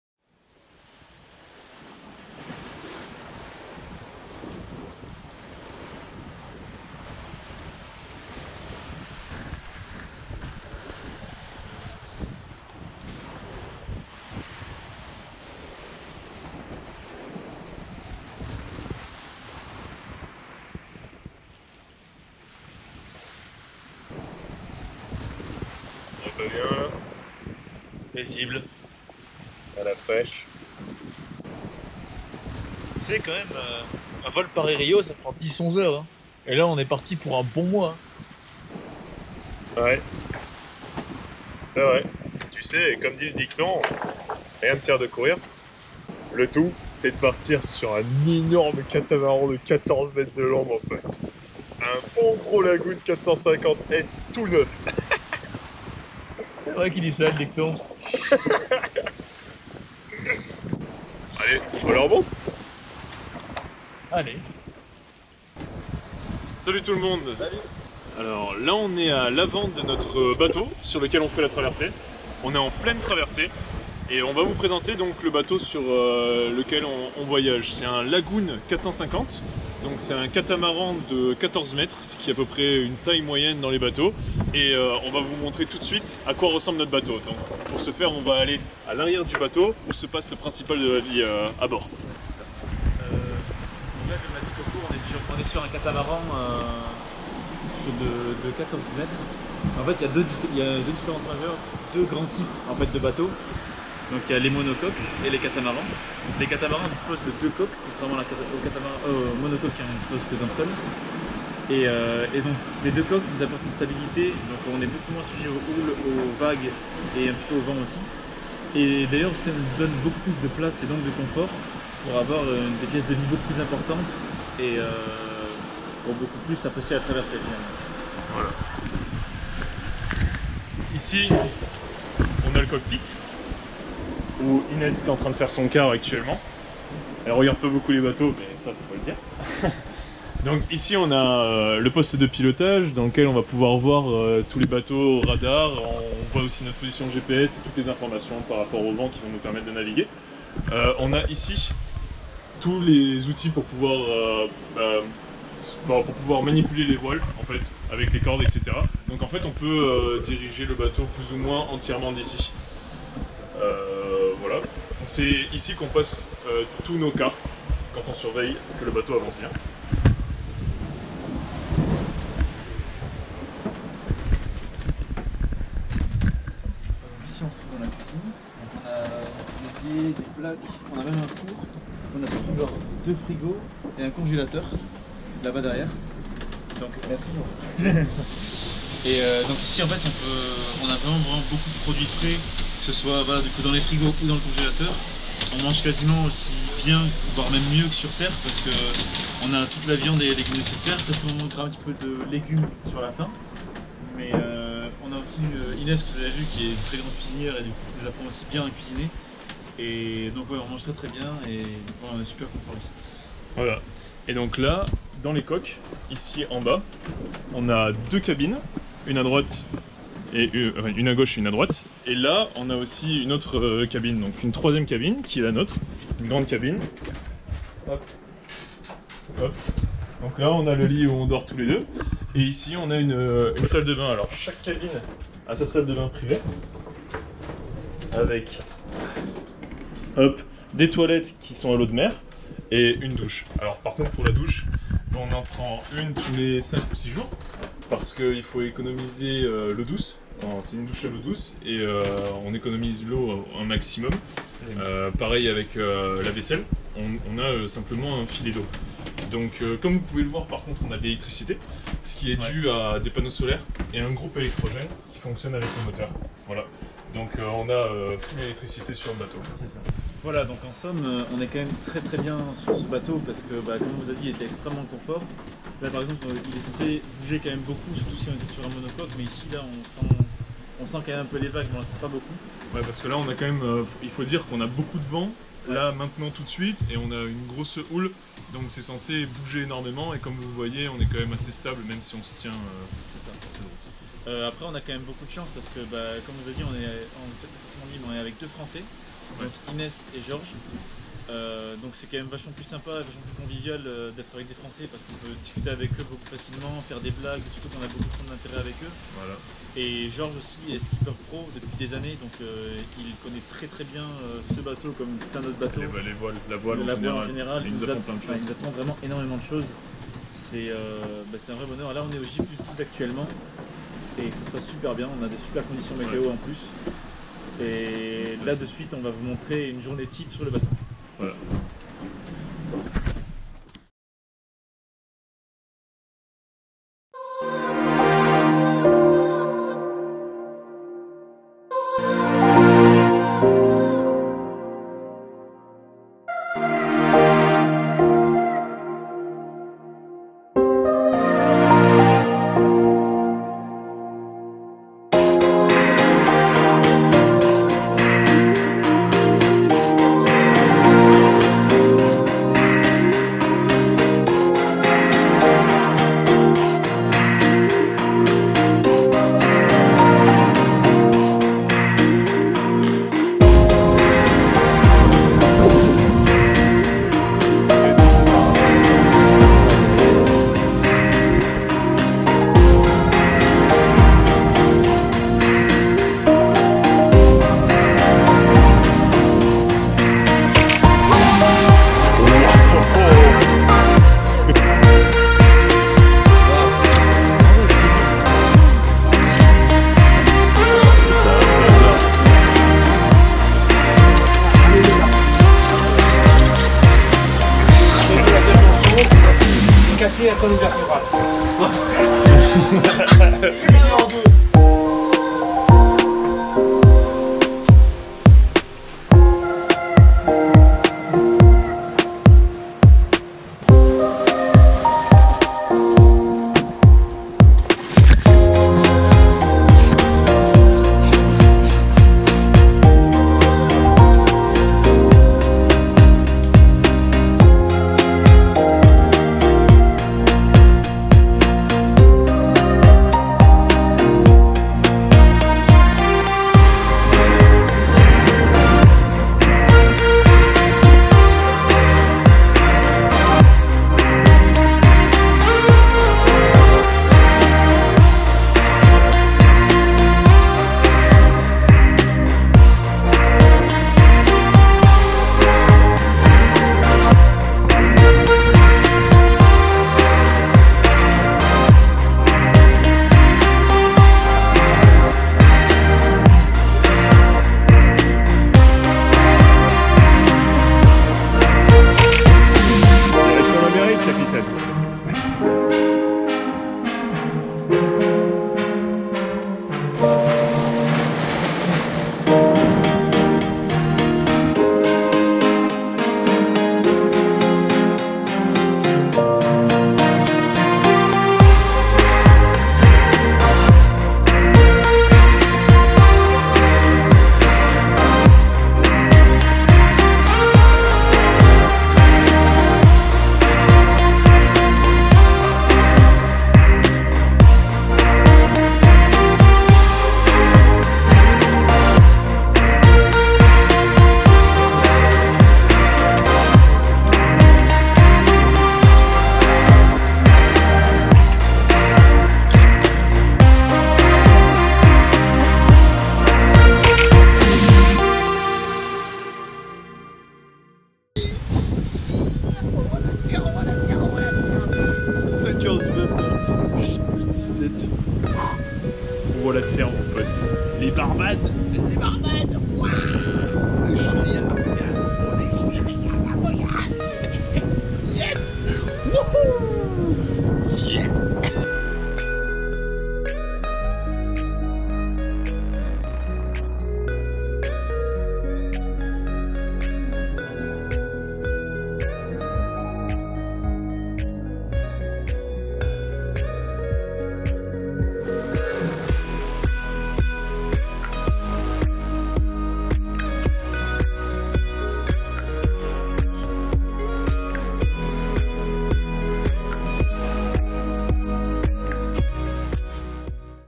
Défi n°5 : Traverser l'Atlantique en voilier partie II
Voici la vidéo de notre traversée de l'atlantique sur un catamaran de 14m en tant qu'équipier.